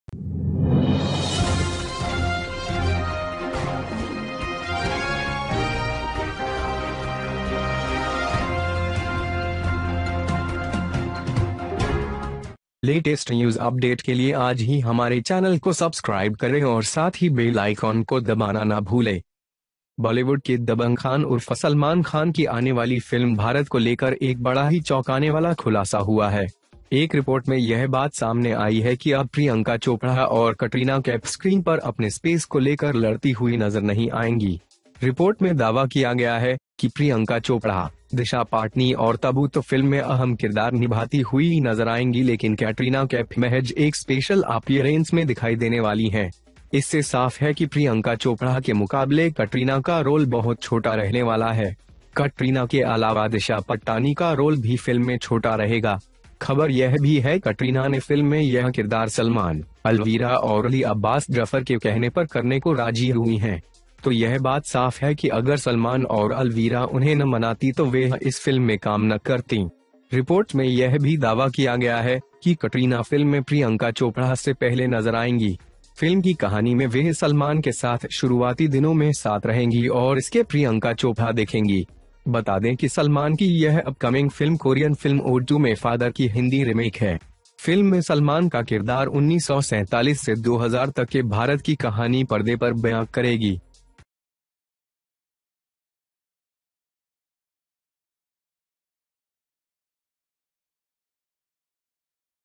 सलमान की 'भारत' को लेकर हुआ चौंकाने वाला खुलासा, देखिये
सलमान की 'भारत' को लेकर हुआ चौंकाने वाला खुलासा, मिन्नतें करने के बाद मानी कटरीना

Salman's 'India' reveals a shocking disclosure, after making concessions, Katrina

There is a big surprise revealing about Bollywood's 'Dabangg Khan' aka Salman Khan's forthcoming film 'India'. A report published on Entertainment site Bollywood Life has revealed that now Priyanka Chopra and Katrina Kaif will not be seen fighting their space on the screen.

It has been claimed in the report that Priyanka Chopra, Direction Patni and Tabu will be seen playing a key role in the film but Katrina Kaif is only going to appear in a special event. It is clear from this that Katrina's role is much smaller than Priyanka Chopra.

Apart from Katrina, direction of Pattani direction will also be small in the film. It is also news that Katrina has agreed to do this in the film on the say of Salman, Alvira and Ali Abbas Zafar. So it is clear that if Salman and Alvira did not celebrate them then they would not work in the film.

It has also been claimed in the report that Katrina will be seen before Priyanka Chopra in the film. In the story of the film, she will be with Salman in the early days and she will look like Priyanka Chopra.

Let's say that this upcoming movie of Salman is the Hindi remake of Korean film 'Ode to My Father'. Salman's character in the film will tell the story of India from 1947 to 2000.

BB NEWS!!